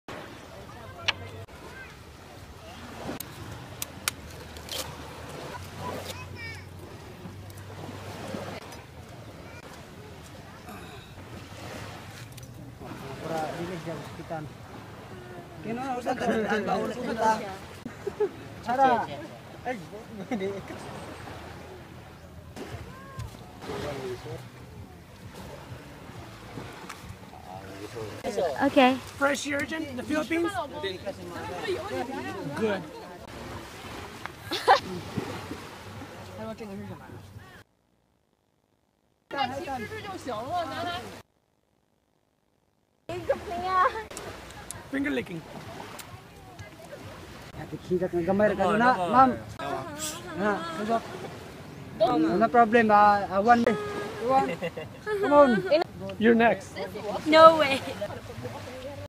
Eating Fresh Sea Urchin in the Philippines
This is the best way to experience fresh sea food and, people's culture... And it doesn't hurt having a wonderful woman by your side